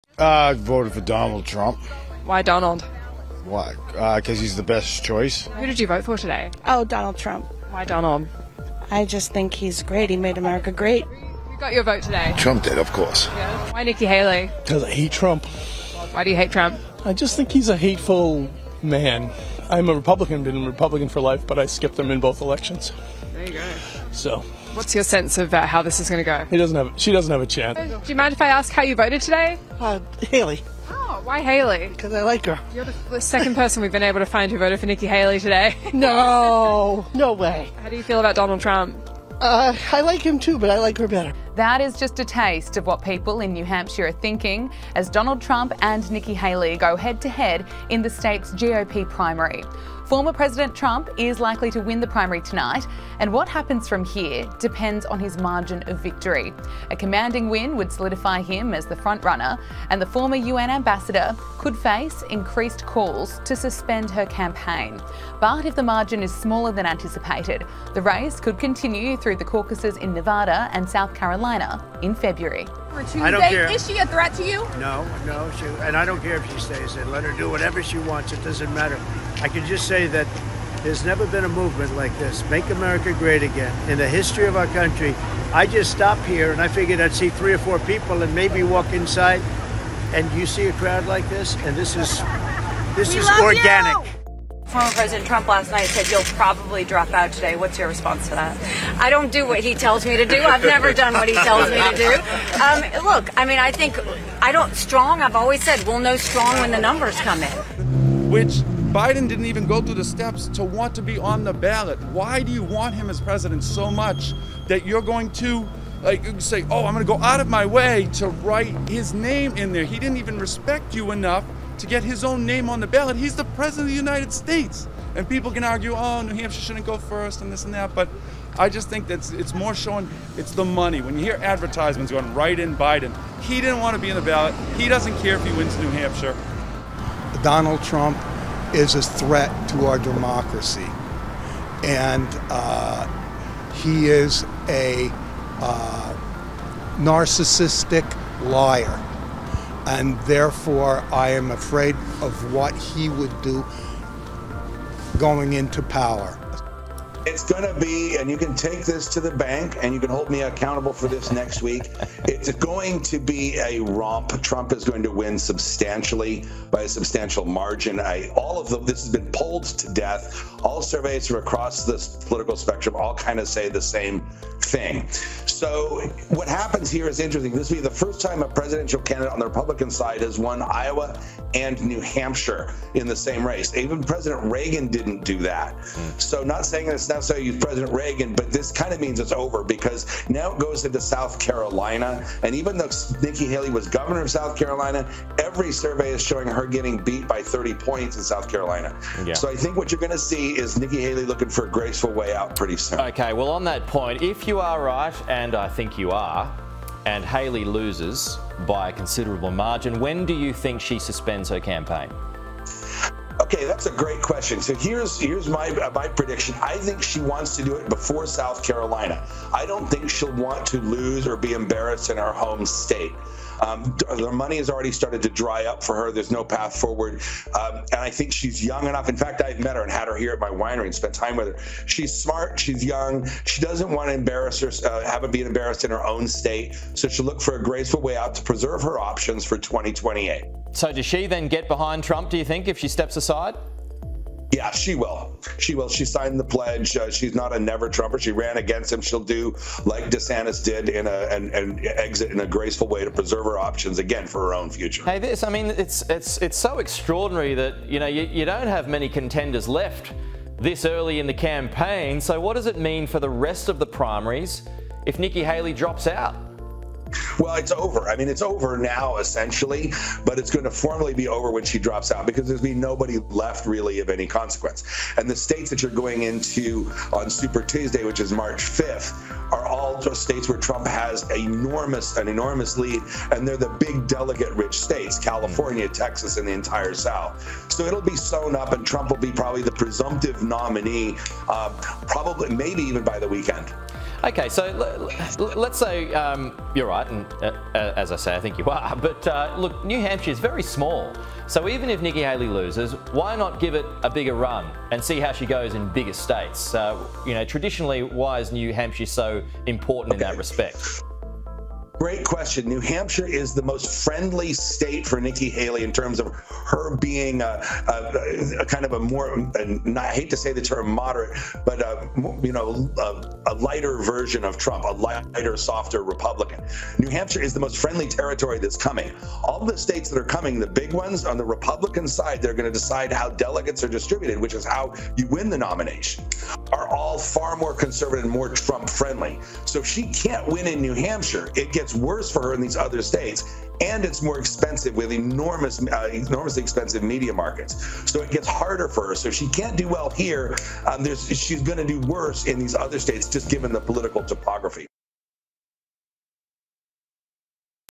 ‘He made America great’: Voters give their verdict as Trump and Haley go head-to-head
Voters in New Hampshire have revealed how they have cast their vote in the state’s GOP Primary.

Donald Trump and Nikki Haley are the only Republican candidates left in this year's race.

One voter told Sky News Australia they voted for the former president because “he’s the best choice”.

“I just think he’s great,” another voter said.

Another voter revealed he was voting to Nikki Haley because he “hates” Donald Trump.